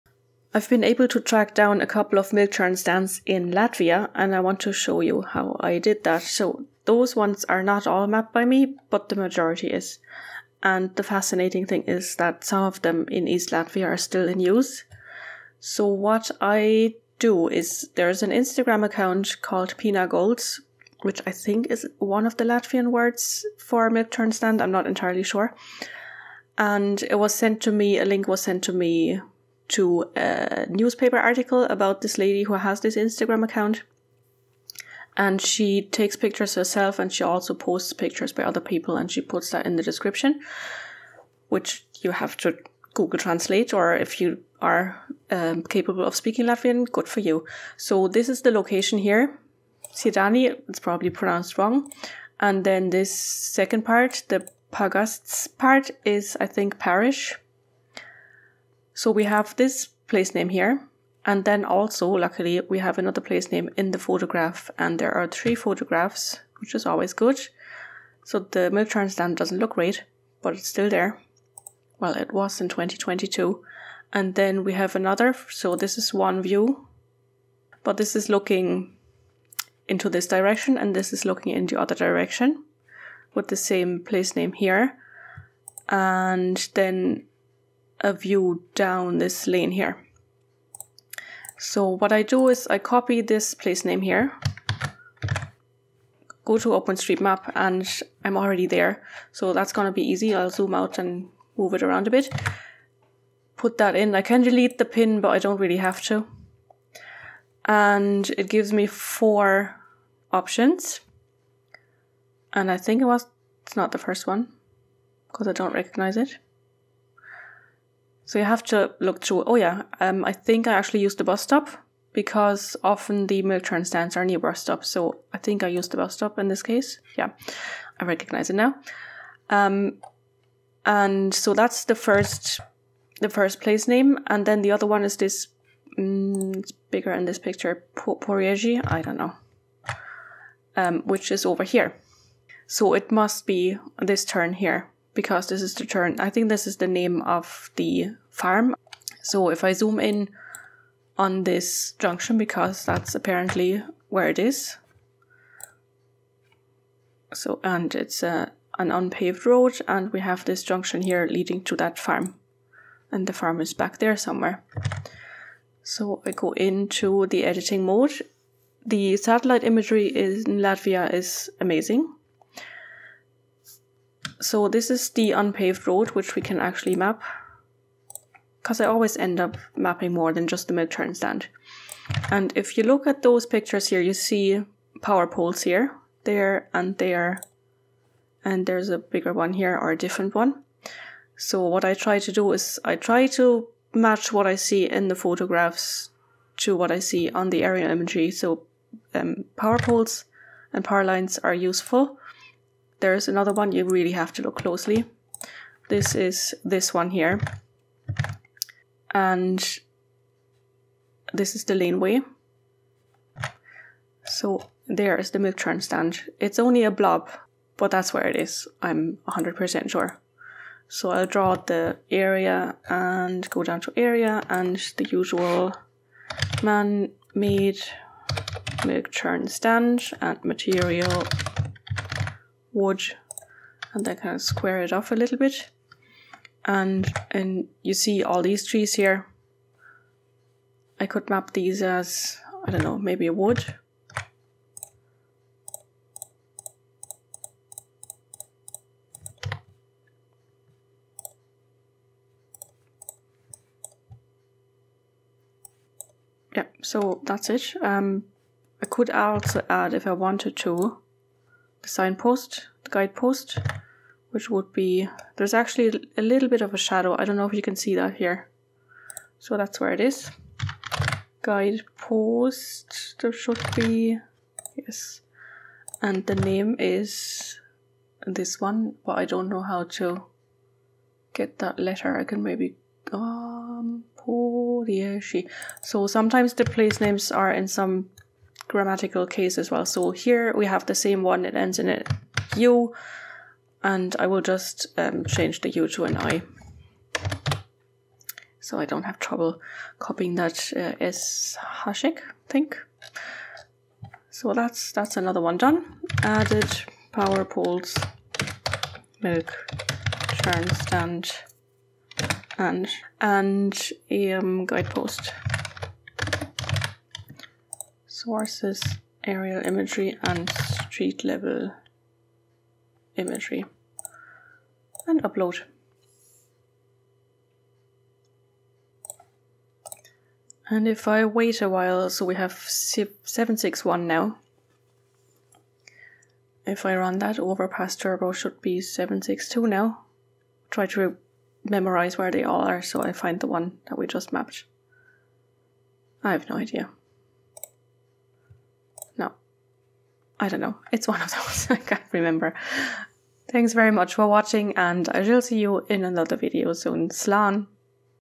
Tracking down milk churn stands in Latvia
With a bit of geo-detective work, it's possible to map milk churn stands abroad, for example in Latvia where some of them are still in use!